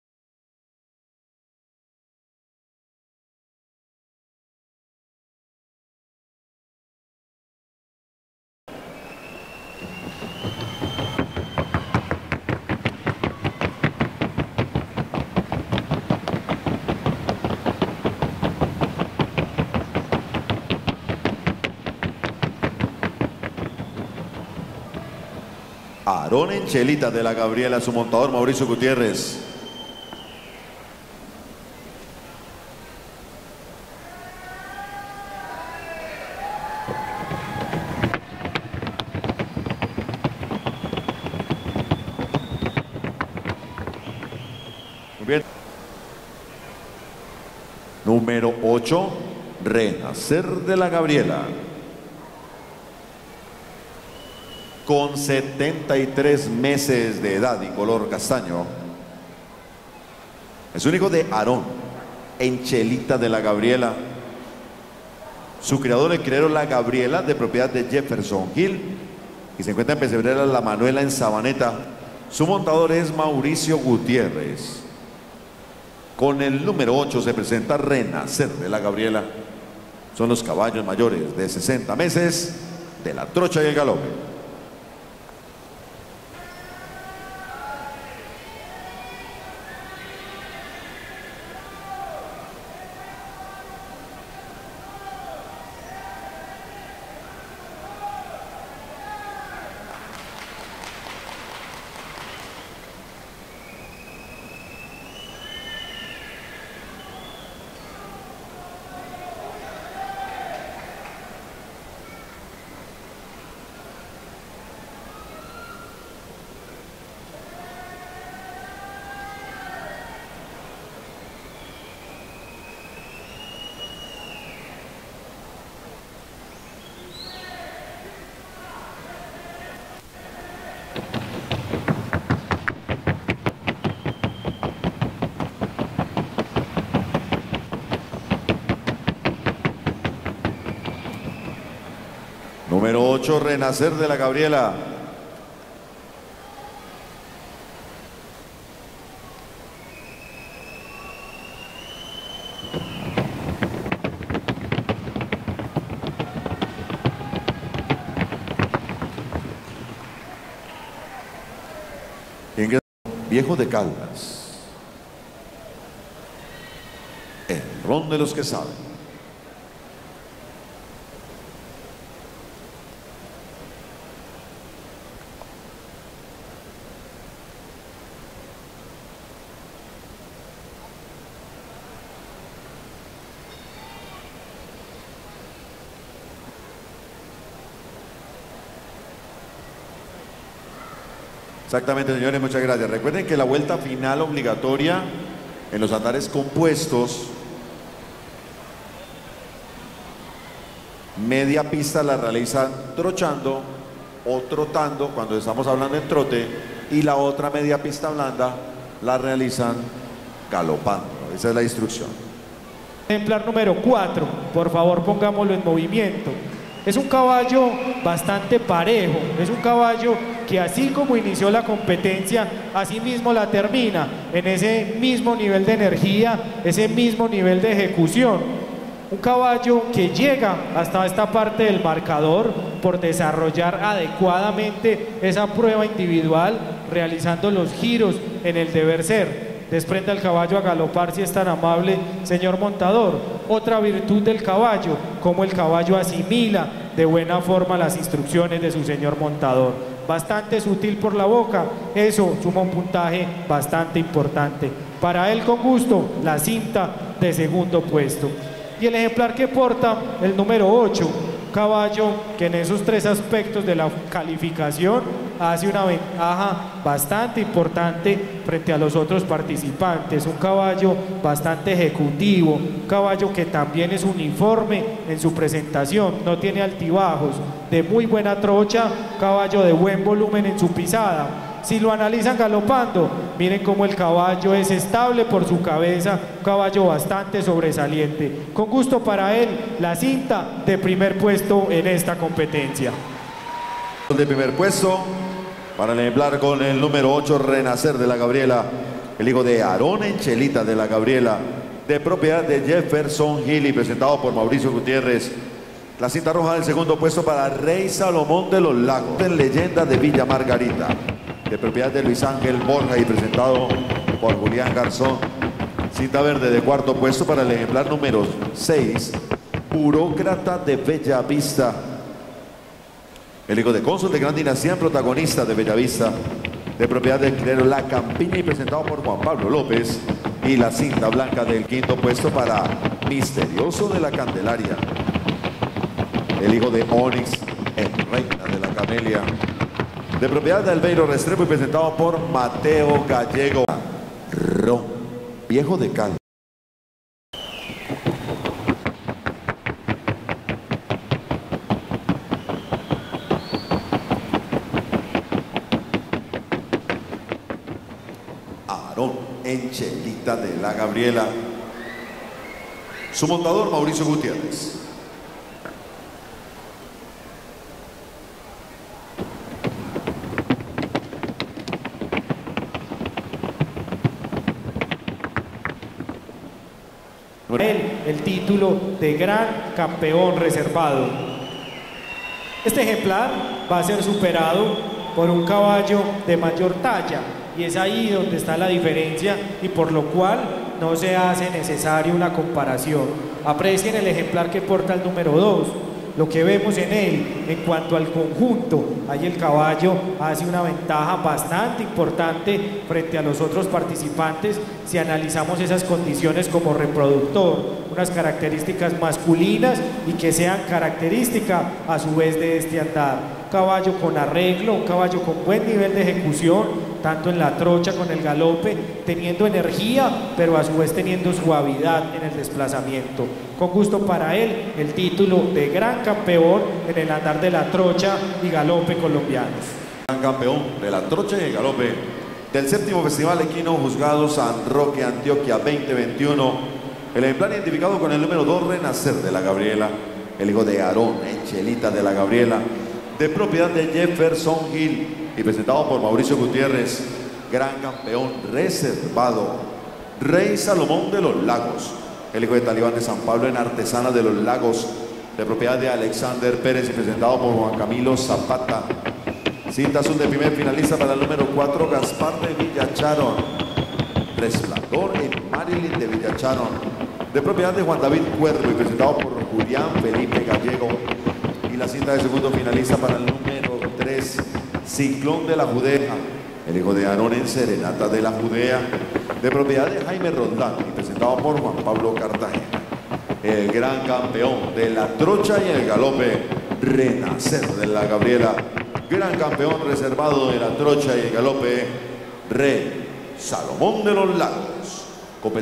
RENACER DE LA GABRIELA, GRAN CAMPEON TROCHA Y GALOPE VII FESTIVAL EQUINO SAN ROQUE ANTIOQUIA 2021
RENACER DE LA GABRIELA Aarón FC x Chelita de La Gabriela ABM Paradigma de La Gabriela PRIMER PUESTO CABALLOS MAYORES DE 60 MESES Y GRAN CAMPEON TROCHA Y GALOPE VII FESTIVAL EQUINO SAN ROQUE ANTIOQUIA 2021